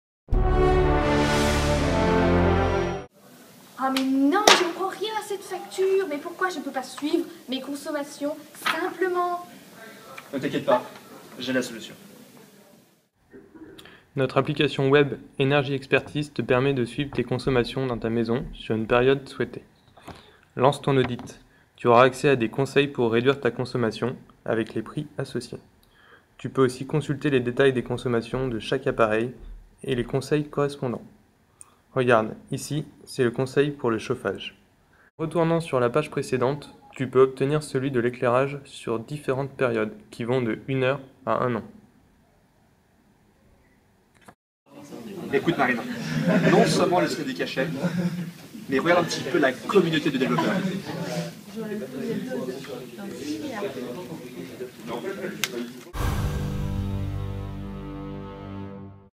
Energy Expertise
Aujourd'hui l'augmentation du coût de la vie, des prix de l'électricité et du gaz notamment, devient un problème pour les consommateurs. Le réchauffement de la planète ont ainsi fait du management de la consommation d'énergie une priorité pour les particuliers.
De plus en plus de sociétés proposent des audits de bâtiments dans le secteur tertiaire mais il est très difficile pour des particuliers, souhaitant être informés de leurs consommations, de suivre ces dernières en temps réel à moindre frais.
Ainsi, en réponse à une demande identifiée des particuliers, nous avons développé une solution web fournissant une expertise sur la consommation en énergie de la maison de l'utilisateur. Le site fournit une interface complète présentant les informations primordiales sur la consommation énergétique d'un habitat, l'utilisateur peut ensuite choisir de générer une analyse de ses consommations et un devis pour les réduire, avec les prix associés.
L'utilisateur devra être muni d'un petit ordinateur le Raspberry Pi grâce auquel il aura son propre serveur chez lui avec ses propres donnés et des capteurs qu'il branchera sur les appareils dont il souhaite suivre la consommation. Ce dernier est disponible à l'achat pour un coût raisonnable.
Nous avons installé sur cet ordinateur un serveur de Tomcat ainsi qu'une base de données Mysql, et enfin un environnement d'exécution Java. Lorsque l'utilisateur lance l'application web, le serveur Tomcat puis la base de données sont lancés, et enfin notre solution est déployée sur le serveur Tomcat et est accessible à partir d'un lien ou d'une adresse Ip. Chaque Raspberry à son adresse Ip fixe. Ces technologies nous garantissent de bonnes performances. La carte joue donc le rôle de serveur et fait le pont entre les capteurs et notre solution.
Nous avons aussi mis en place un autre site web pour valoriser notre projet ainsi que les projets de l'ECE. En effet nous avons choisi la valorisation Open Source pour notre projet. Ce deuxième site se veut plus généraliste il présente les différents projets de l'ECE et nous permettra d'y publier les codes sources des projets ainsi que des tutoriels pour utiliser nos solutions.
L'ensemble des solutions que nous avons mises en place permet à un utilisateur d'obtenir un suivi correct de ces consommations, ainsi que des conseils concrets et réalisables.